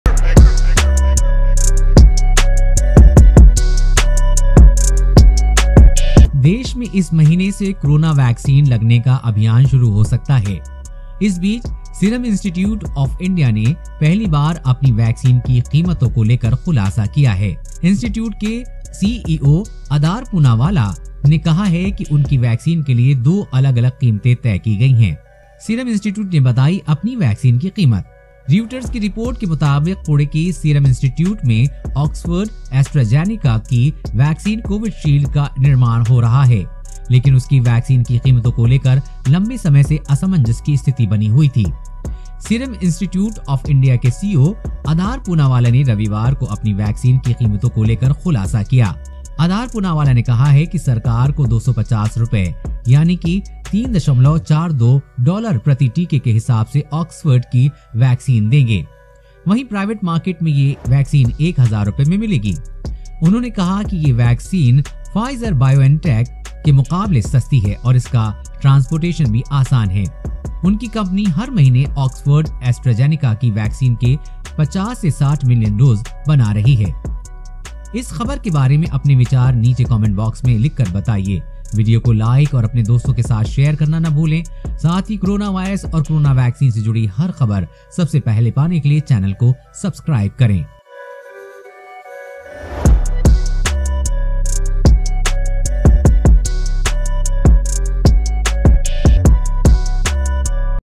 COVID19 Vaccine Price: वैक्सीन के लिए देना होगा इतना पैसा! सीरम इंस्टिट्यूट ने किया कीमतो का खुलासा!
COVID19 Vaccine Price: Will Pay So Much Money For Vaccine! Serum Institute Revealed The Price! Corona Virus Vaccine Latest News And Updates

EDITOR ►Shadik